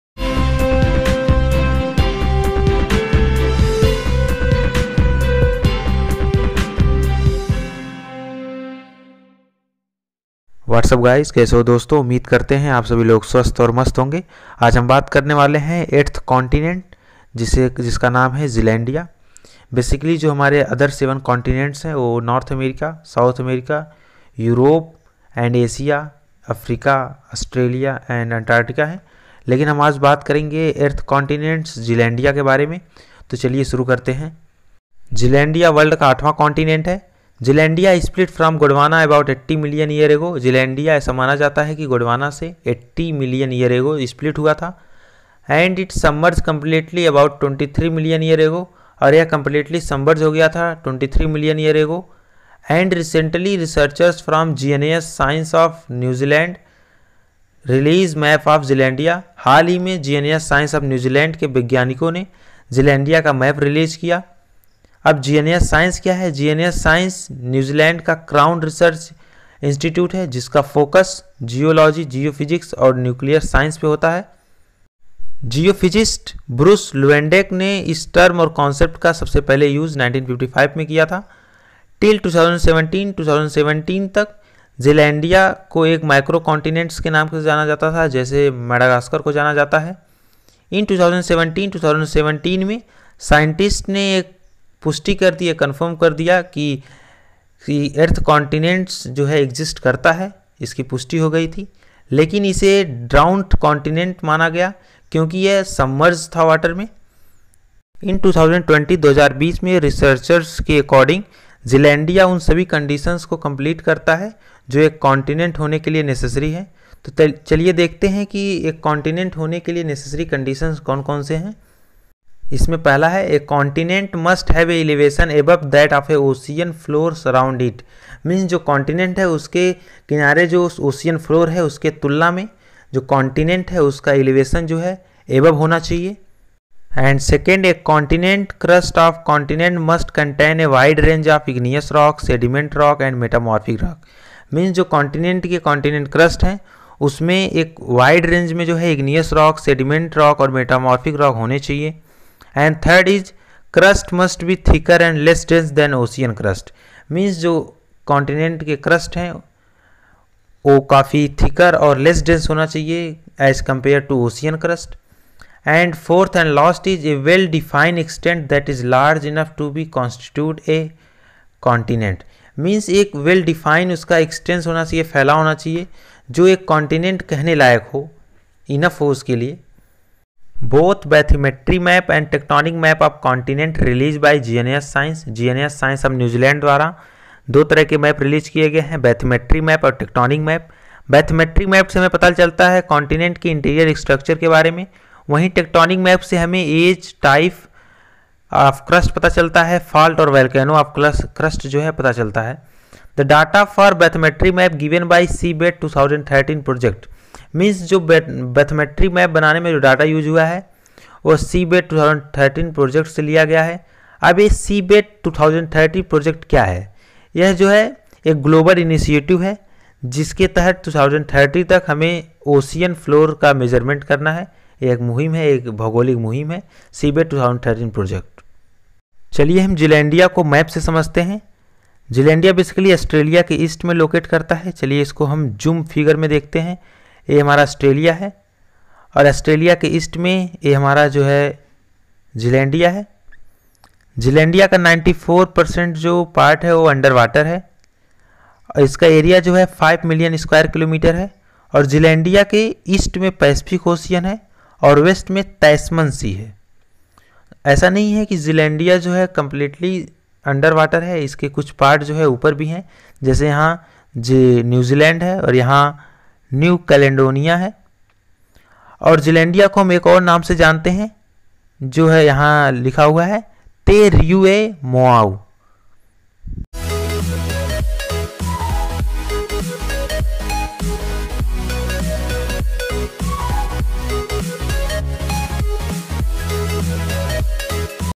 Zealandia: world 8th continent// zealandia /
In this video, I have done an explanation about "zealandia : world  8th continent. "
Recently, researchers release the map of zealandia.